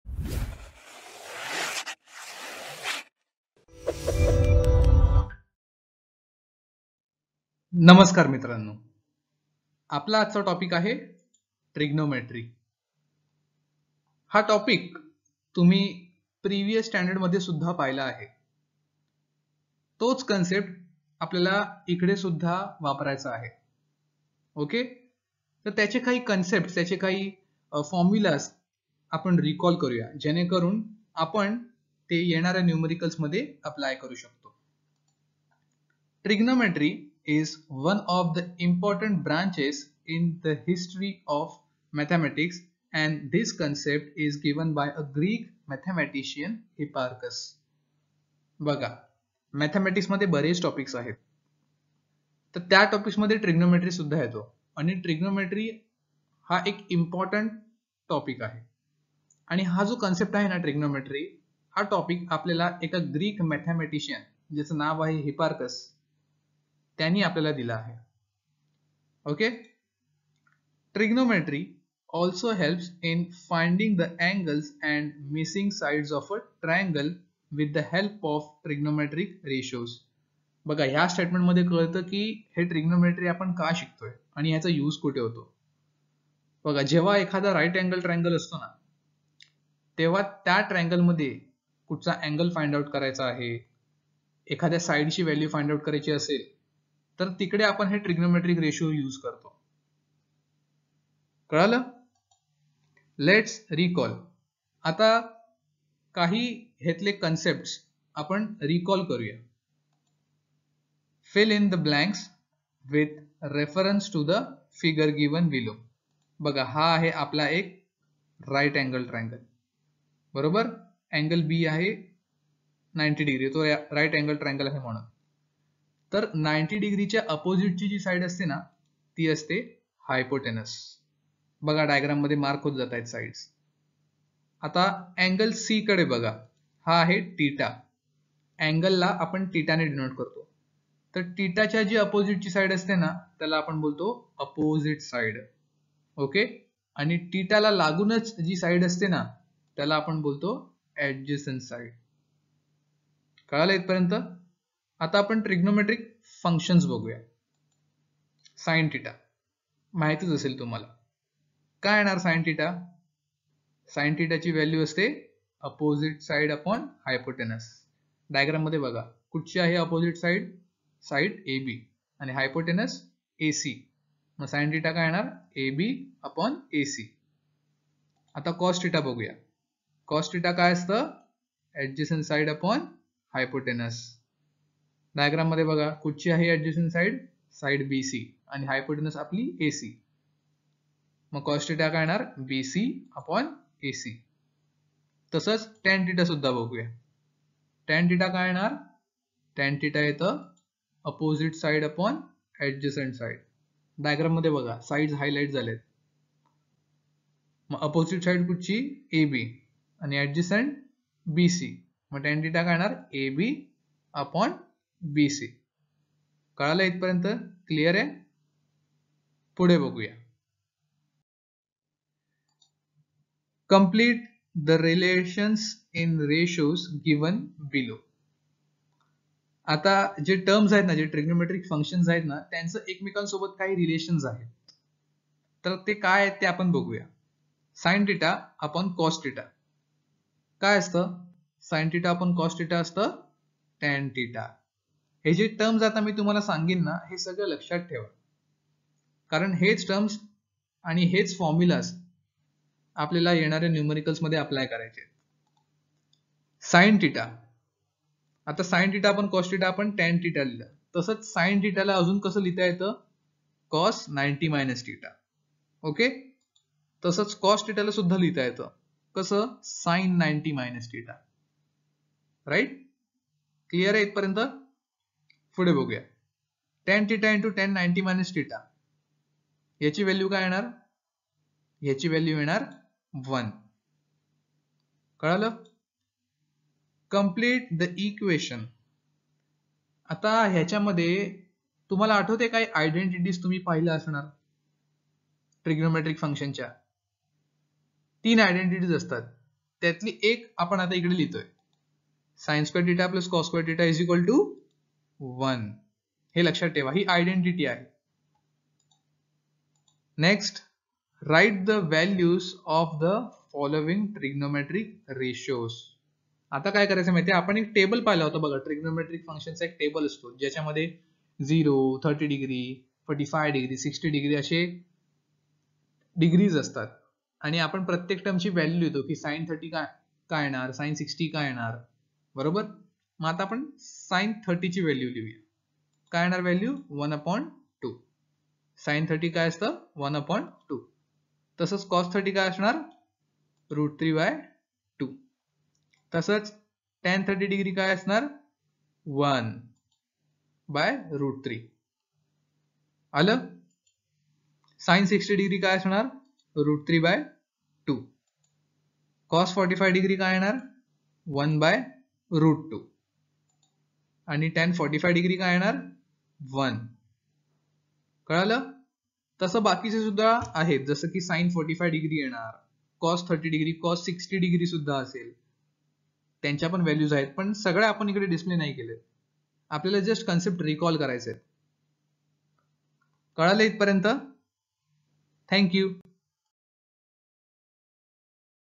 Basics of Trigonometry | 10th SSC | Maharashtra Semi-English | Alphy
In this video, we walk you through what is Trigonometry and basics of Trigonometry. Ever wondered what Sin, Cos and Tan mean? Who gave us the concept of Trigonometry?